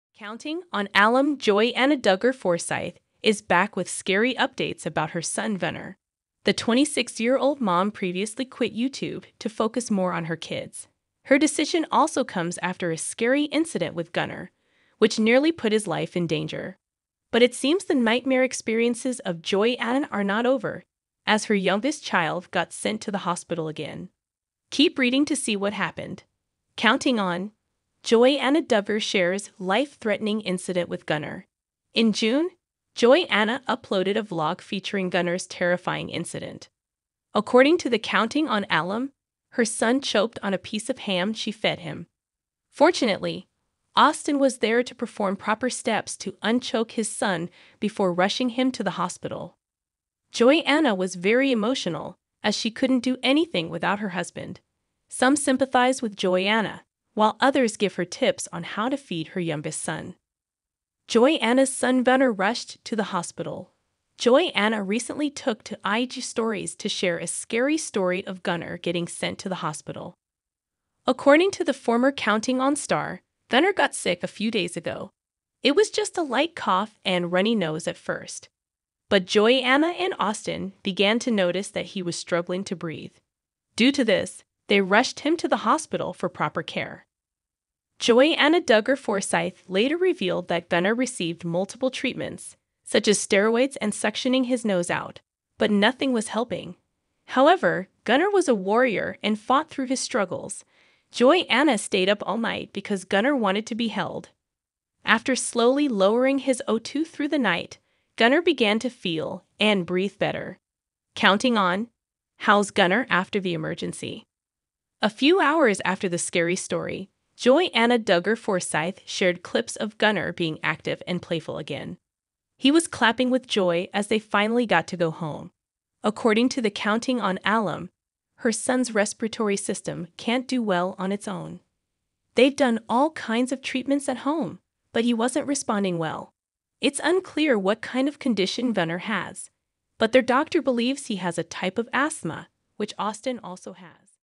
‘Counting On’ Joy-Anna Duggar’s Son Gunner Rushed To Hospital
duggar family,
duggar family documentary,
duggar family news,
duggar family update,
duggar family scandal,